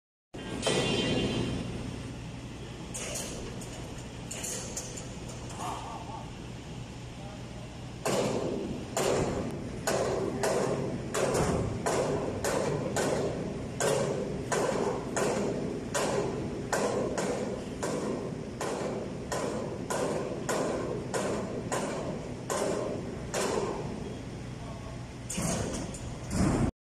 Daswell concrete mixer with pump is ready for shipping to Indonesia.
The use of a large cubic mixer greatly improves construction efficiency and service life.